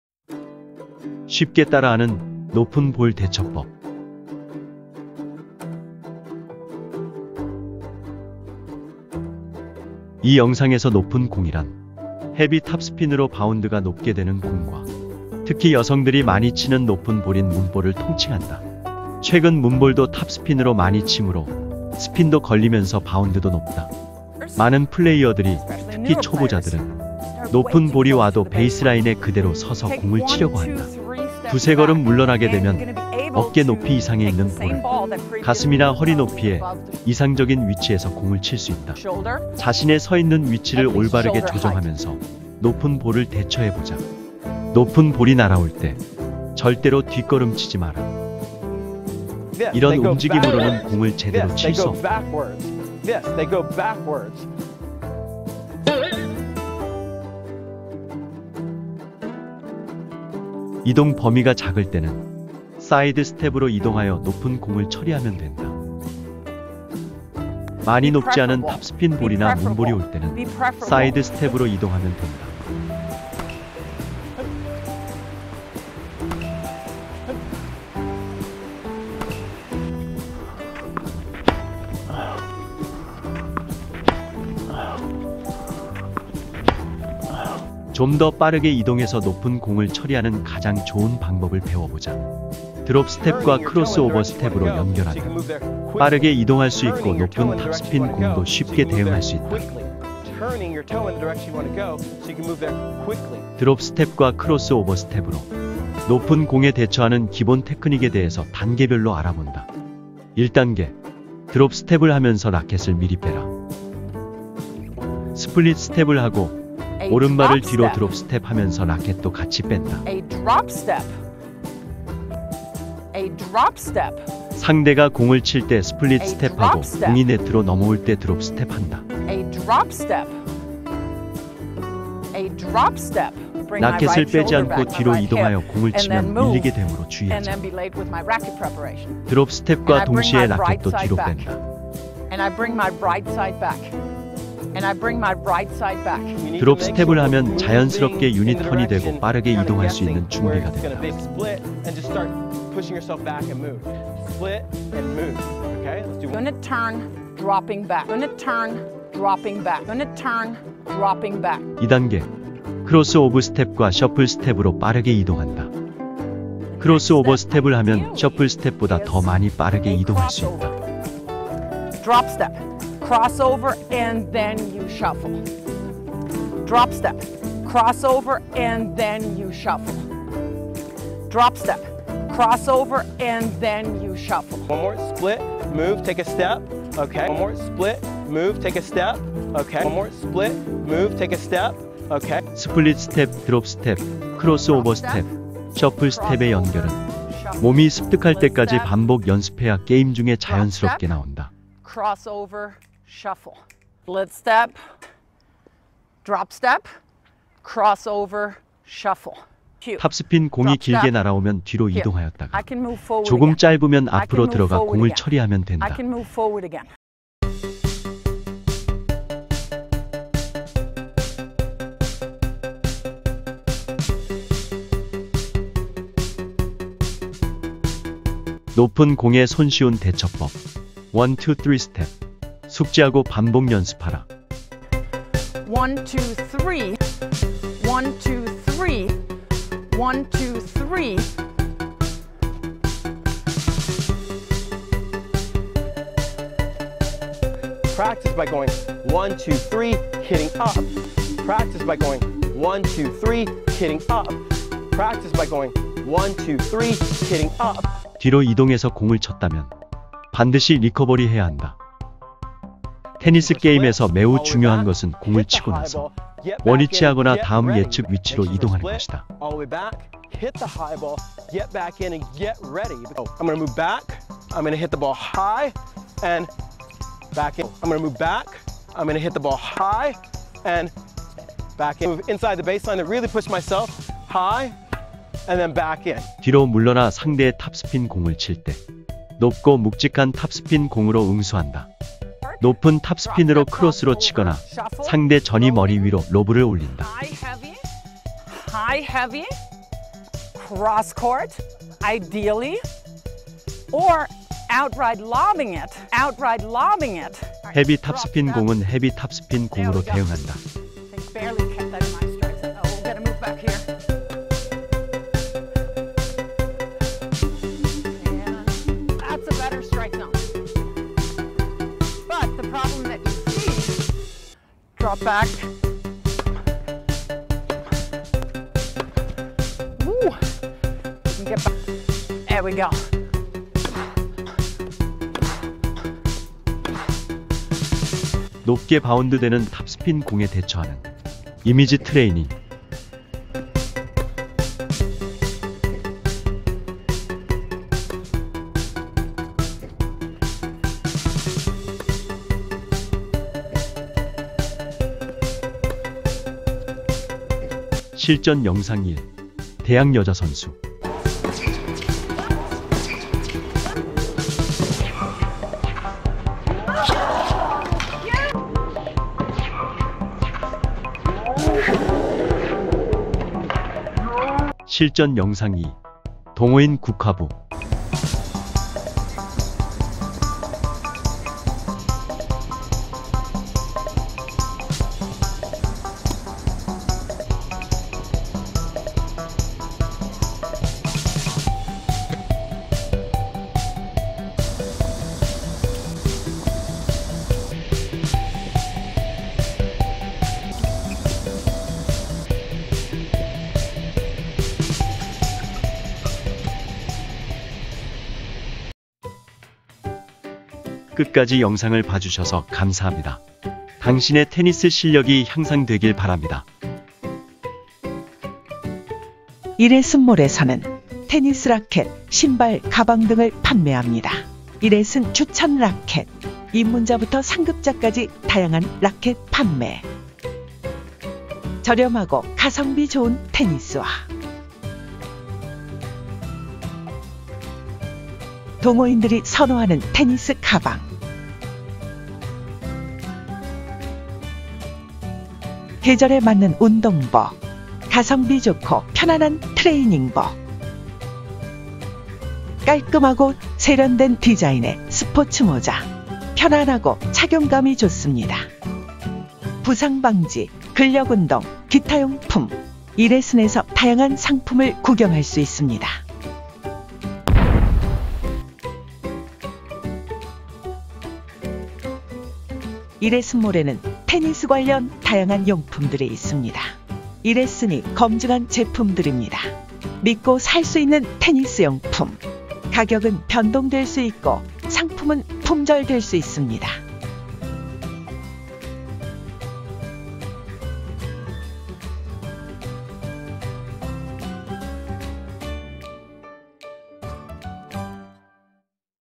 [테니스] 쉽게 따라하는 높은 볼 대처법
많은 플레이어들이 특히 초보자들은
높은 볼이 와도 베이스라인에
그대로 서서 공을 치려고 한다.
두세 걸음 물러나게 되면
어깨 높이 이상에 있는 볼을 가슴이나
허리 높이의 이상적인 위치에서
공을 칠 수 있다.
자신의 서 있는 위치를 올바르게
조정하면서 높은 볼을 대처해 보자.

▶이레슨 후원 (토스뱅크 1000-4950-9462 최근우)